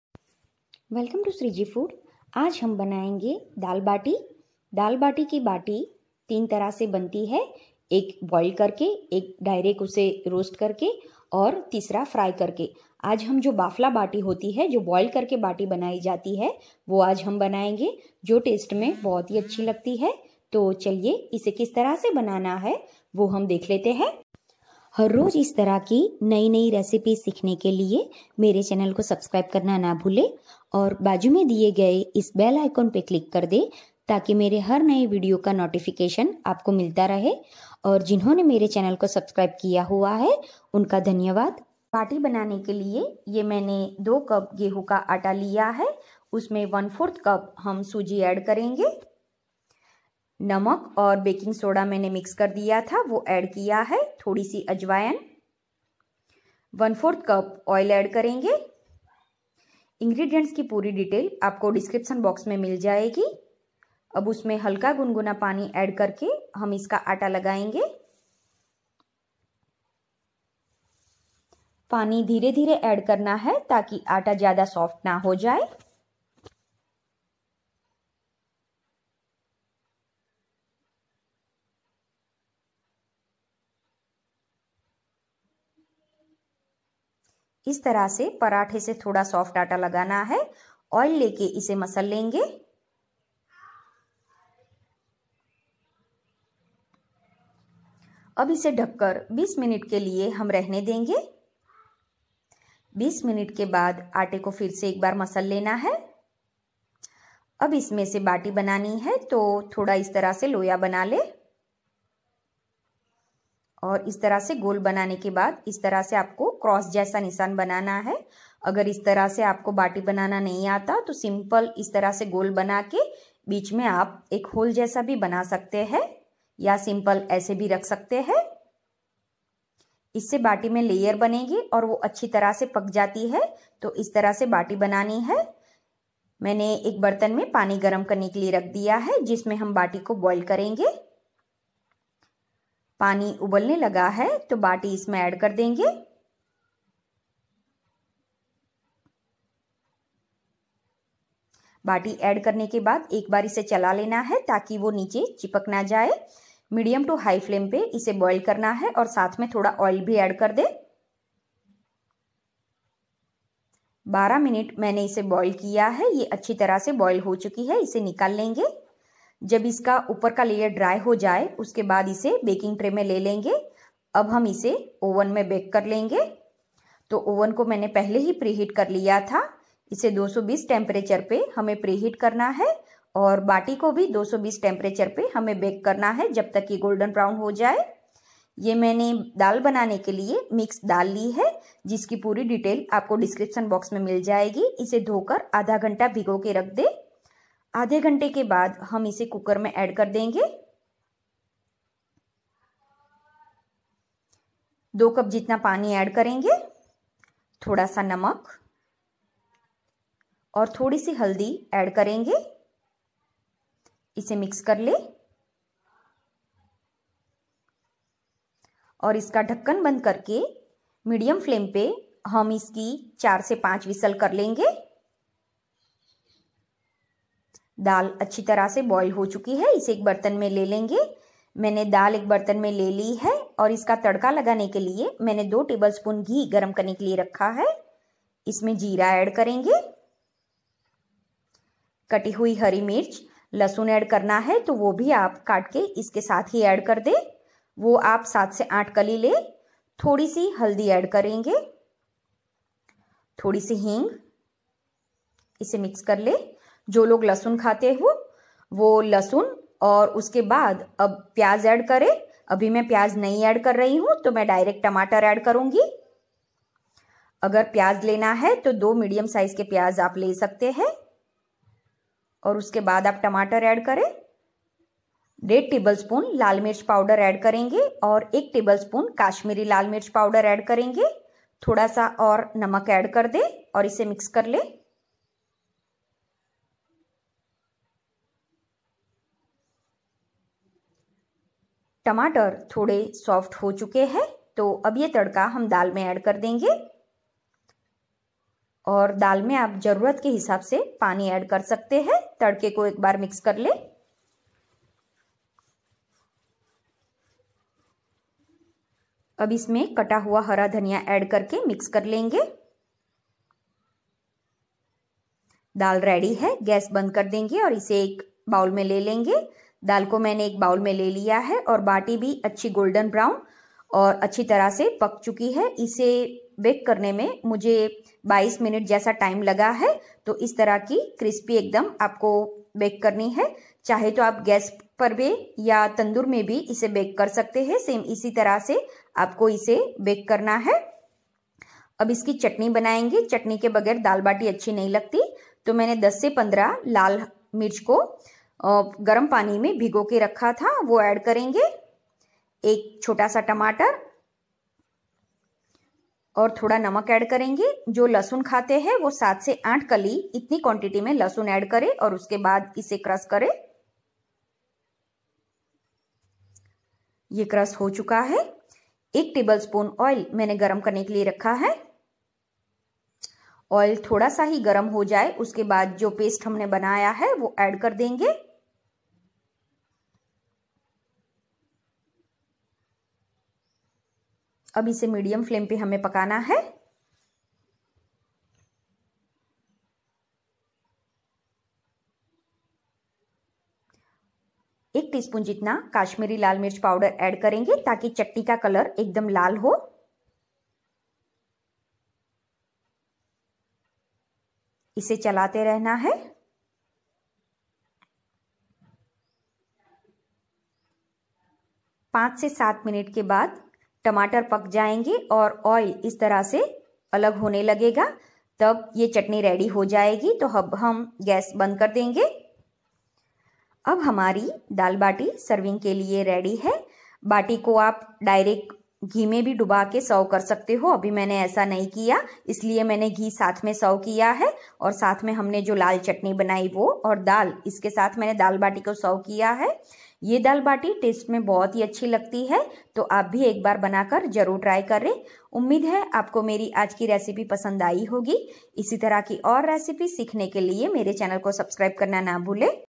दाल बाटी बनाने की विधि | Rajasthani Bafla Bati Recipe in Hindi
Hello friends today i made Rajshthani food "dal bati ".it calls bafla bati also

ingredients
For bati :
2 cup - wheat flour
1/4 cup - sooji (semolina)
pinch of soda
salt to taste
1ts - carom seeds
1/4c - oil
fennel seeds (optional)
warm water

For dal:
1/2c - yellow moong dal
2tb - green moong dal
2tb - chana dal
2tb - toor dal
2tb - pure ghee
2 - tomatoes
1ts - jeera
1/4ts - Asafatida
2 - green chilli
1-1/2tb - red chilli powder
1tb - kashmiri red chilli
salt to taste
coriander leaves
water as needed
turmeric powder
2 - onions (if you wish)
6-7 - garlic

For chutney :
10-15 - dry red chilli
1 - tomato
7-8 - garlic (if you wish)
1ts - kashmiri red Chilli
salt to taste
1tb - oil